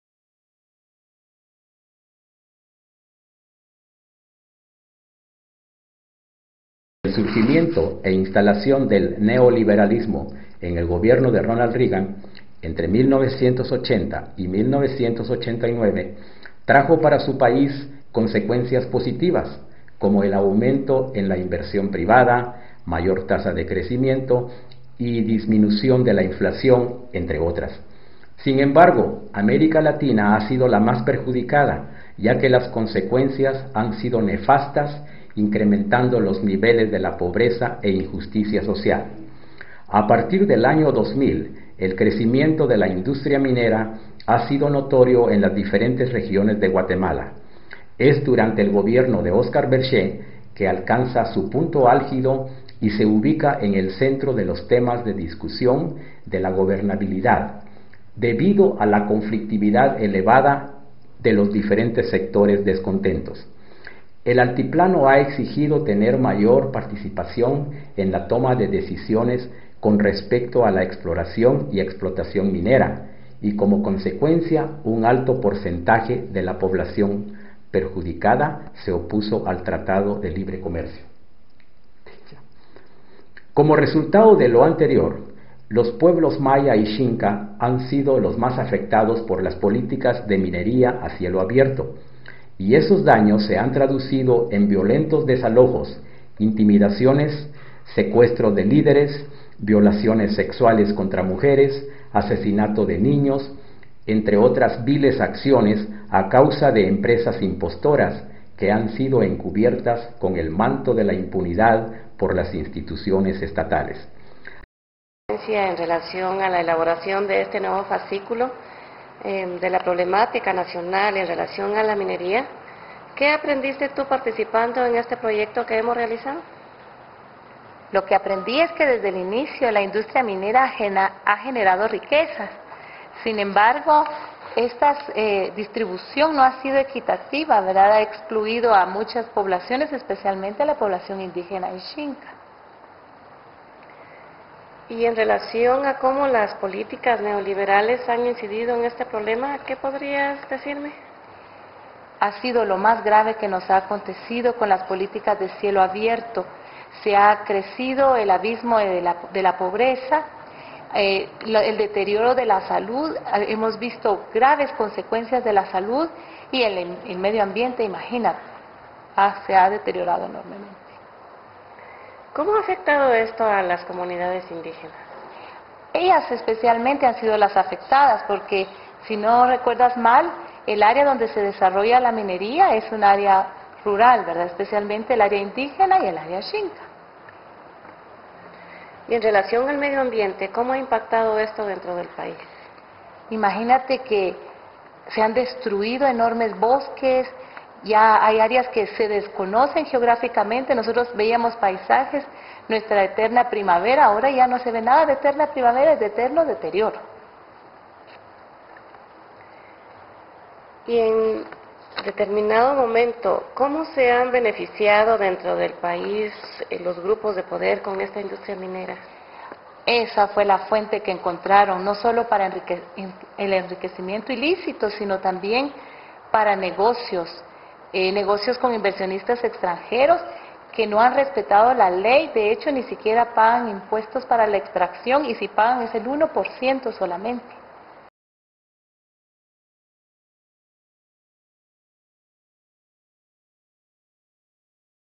Learn how Mining Industry is Devasting Guatemala
is including in our Spanish curriculum how mining in Guatemala is making big corporations in Canada and USA richer while leaving entire communities in Guatemala devastated by the contamination.